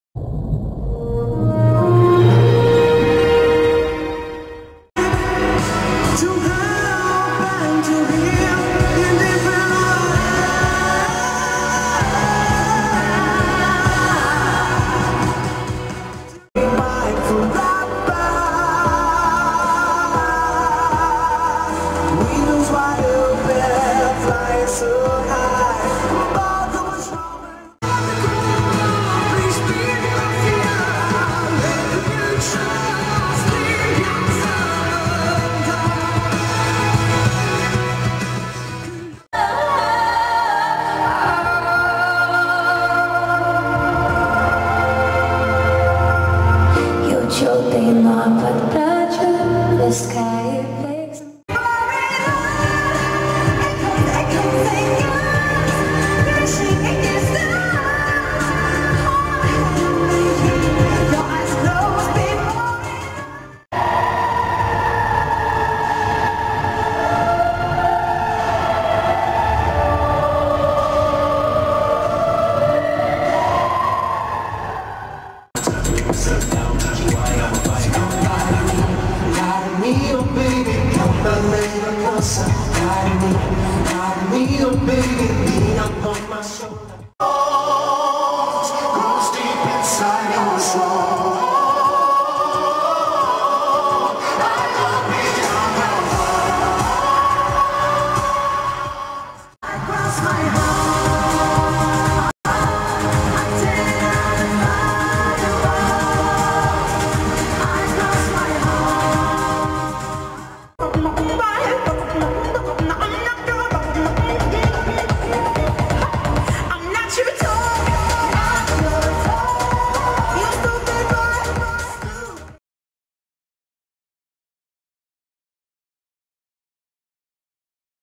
1 DAY REHEARSALS || MY TOP 10 || EUROVISION 2018
Hii!! This is my first video of Eurovision 2018 because I was very busy :( but I'll do more videos this week. Hope you like this, it's based on my opinion so respect it please.
Thanks !!